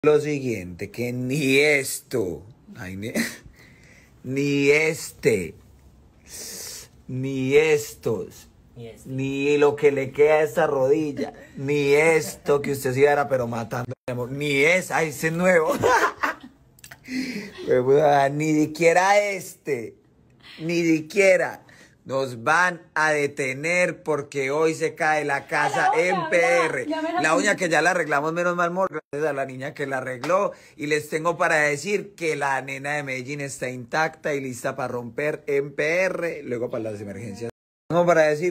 KAROL G MUESTRA SUS HERID4S DE LA CAÍDA
KAROL G quedo muy afectada y golpeada